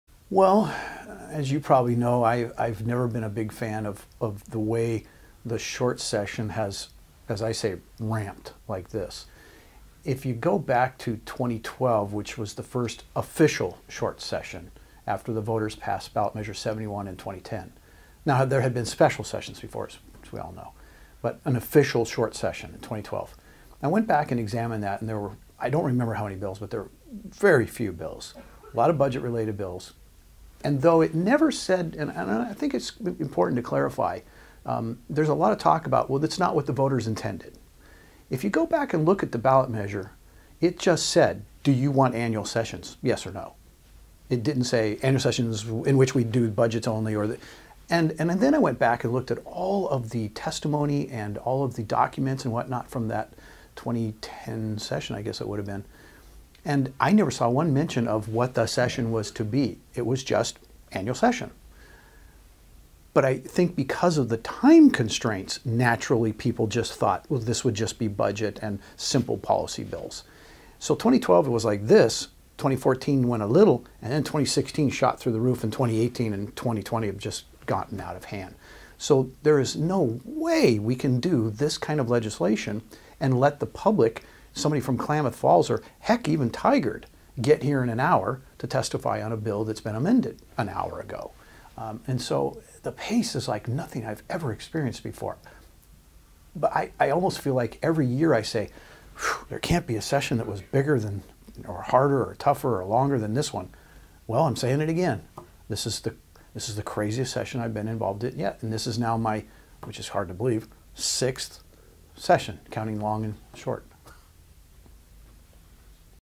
Rep. Post: 'This is the craziest session I've been involved in'
Oregon state Rep. Bill Post, R-Keizer, talks with me in his office on Feb. 21, 2020.